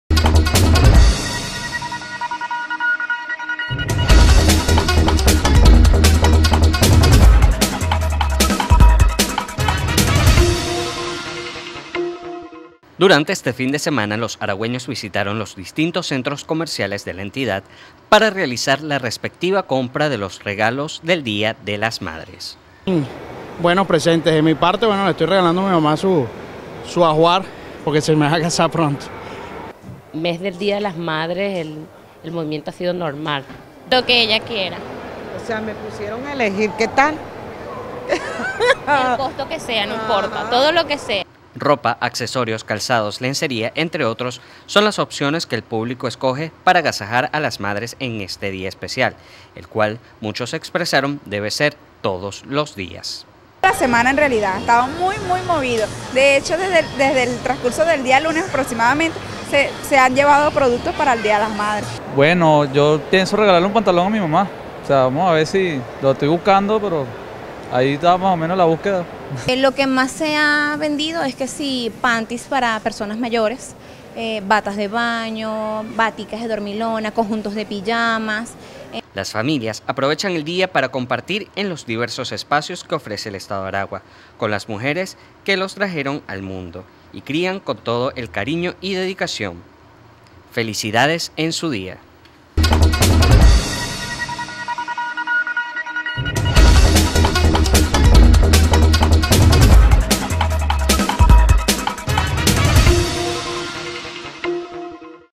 Aragüeños colmaron centros comerciales para comprar regalos a las madres 11-05-2014
Durante un recorrido el equipo de TeleAragua los aragüeños colmaron centros comerciales para comprar regalos a las madres